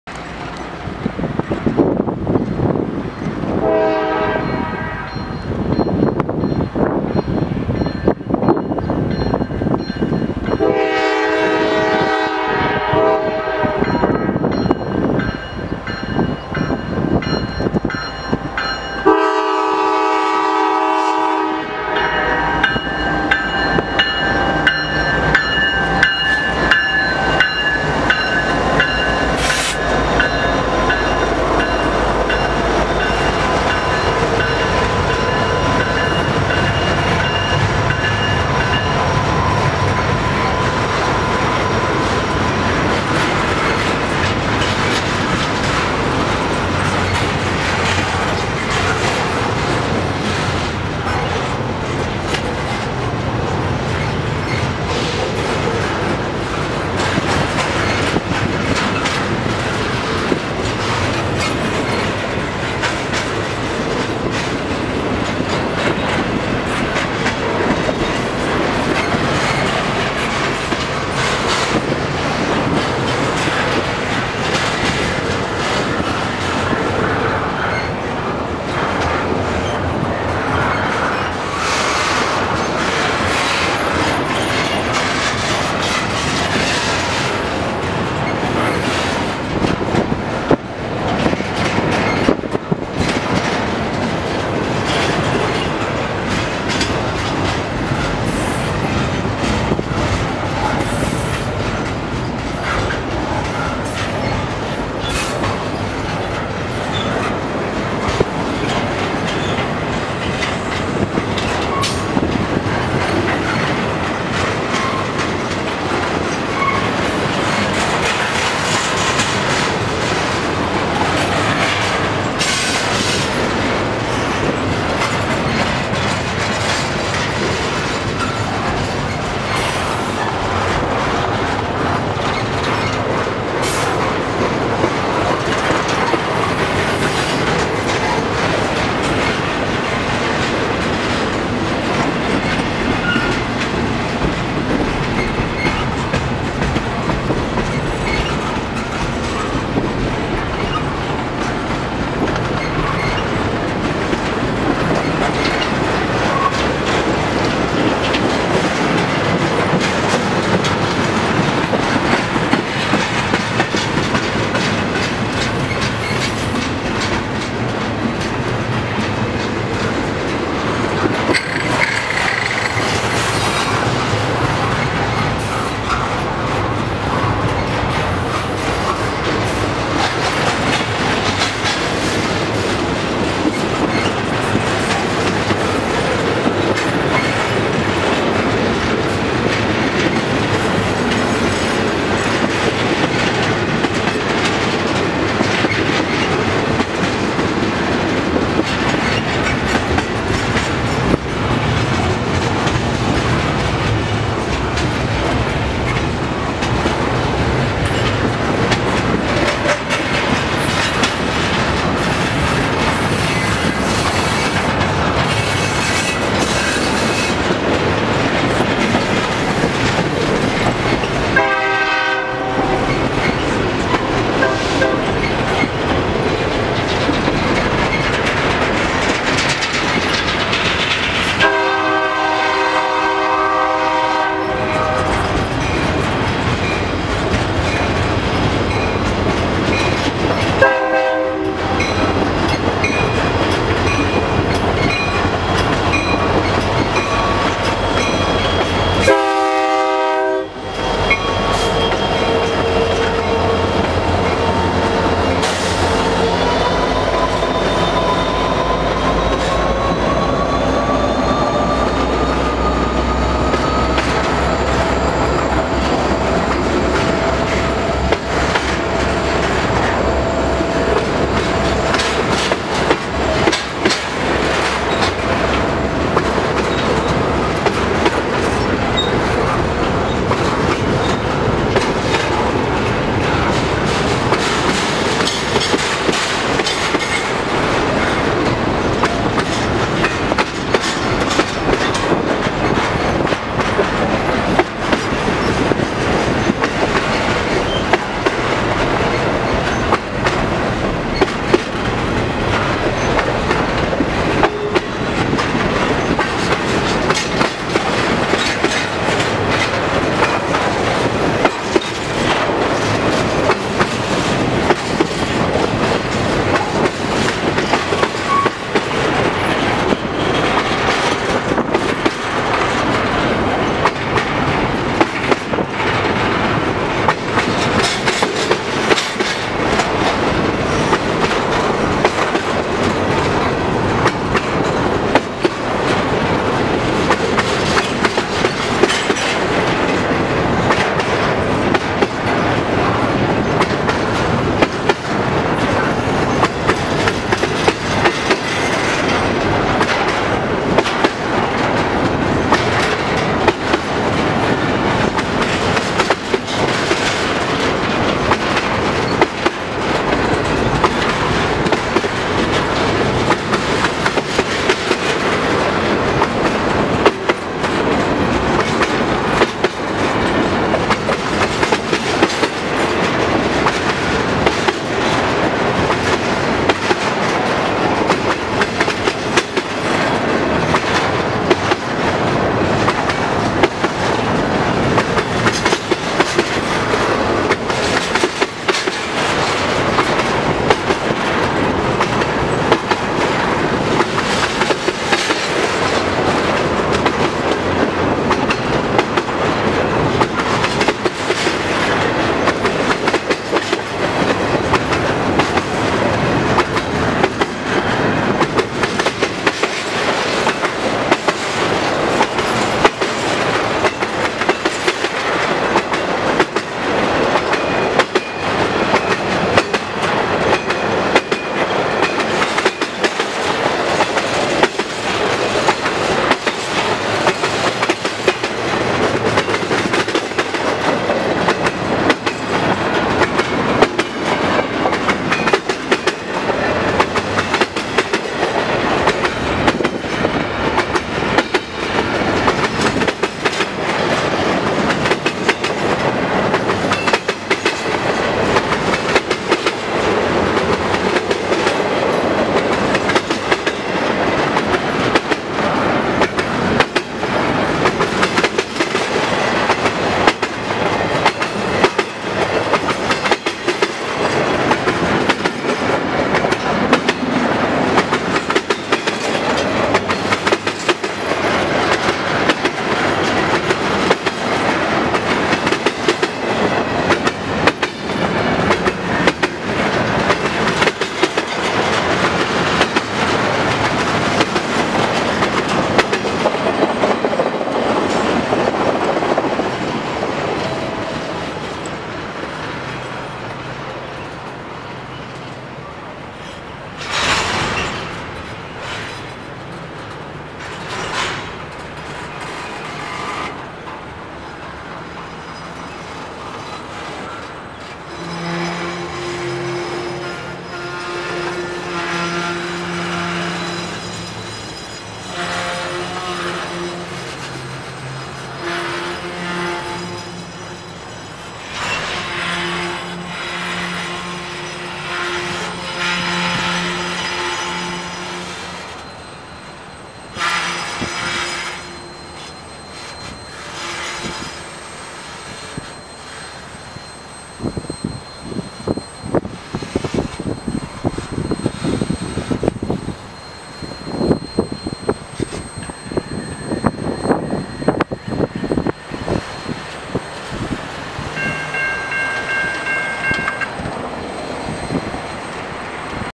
MEET: NS C44-9W & SD60M lead a WB Intermodal while a UP SD70M & NS SD60E lead an EB Intermodal
NS 9856 (C44-9W) & NS 6796/ex-CR 5558 (SD60M).
UP 5199 (SD70M) & NS 6906/ex-HLCX/UP/C&NW (SD60E). 93 Rail Cars. Shot in Toledo, OH on the NS Chicago Line.